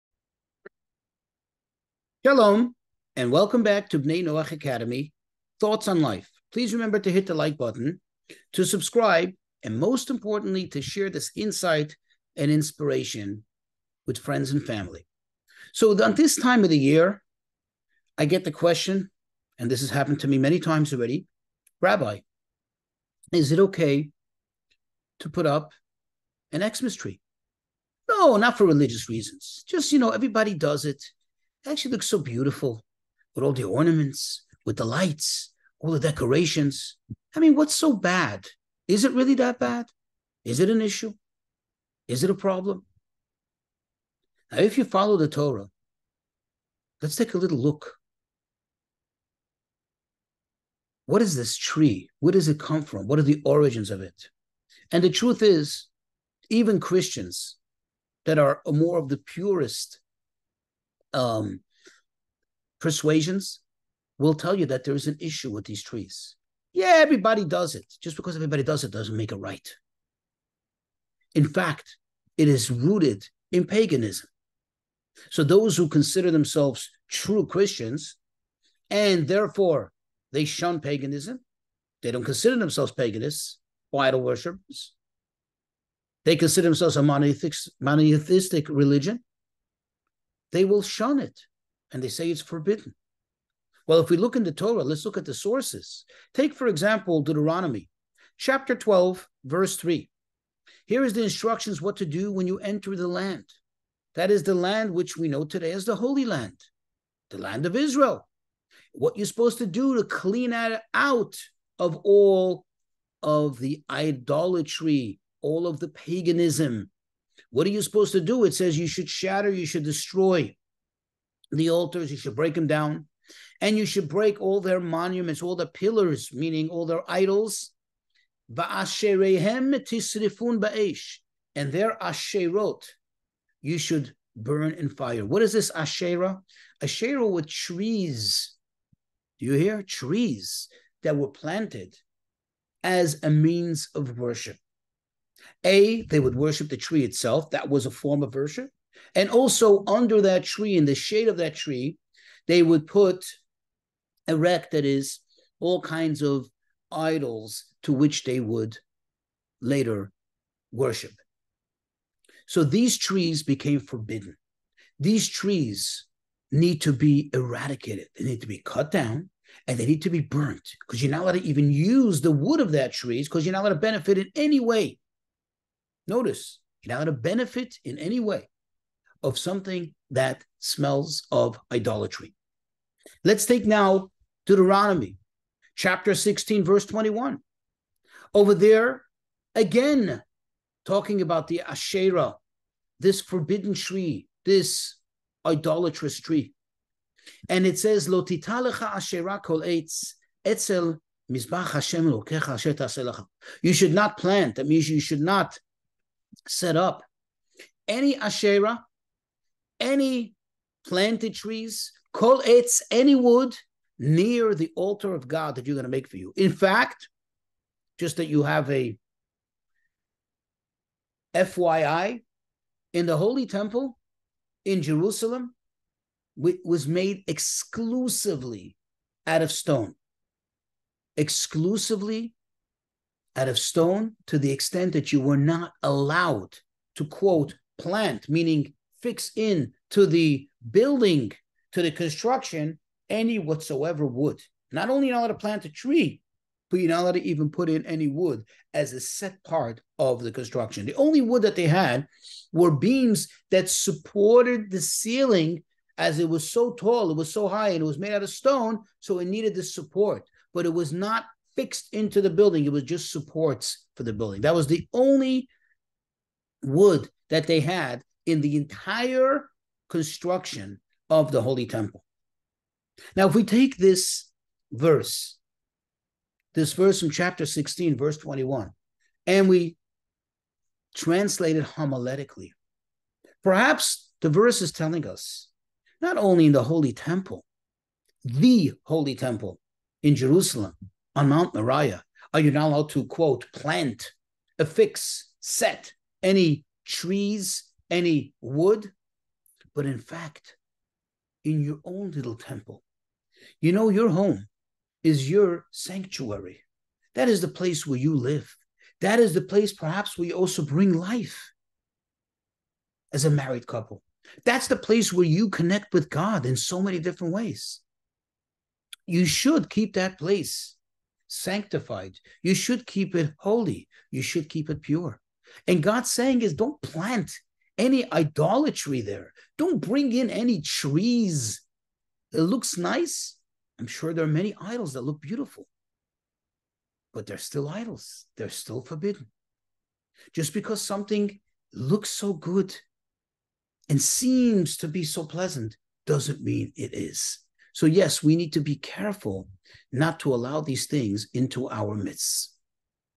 The Problem With X-mass Trees. By: Rabbi Avrohom Brashevitzky
Based of Deuteronomy12:3 and 16:21. Is it ok to put up a "holiday tree", not as a religious symbol, rather as a decoration?